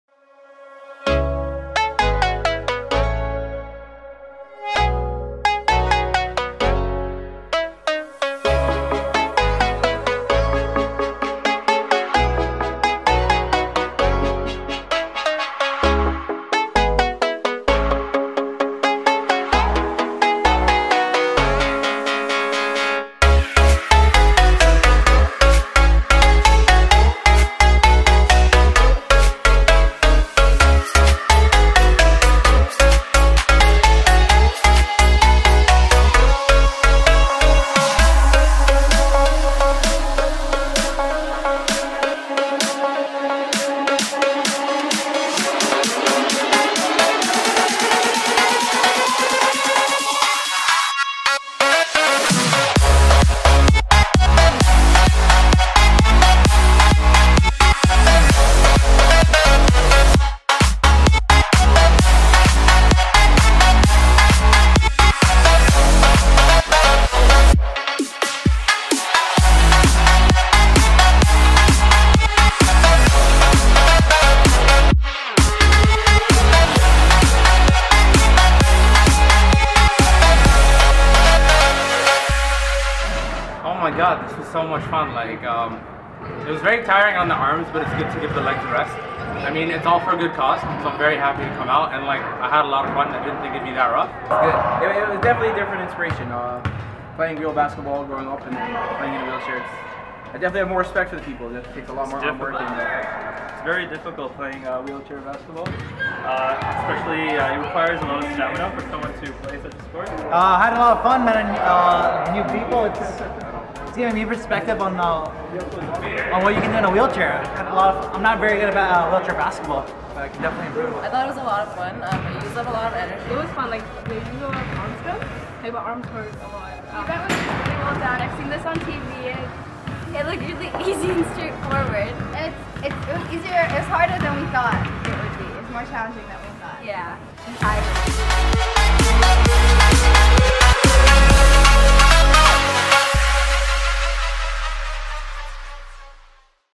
Tetra Ryerson Presents Wheelchair Basketball (Creating Awareness)
Tetra Ryerson basketball event to create awareness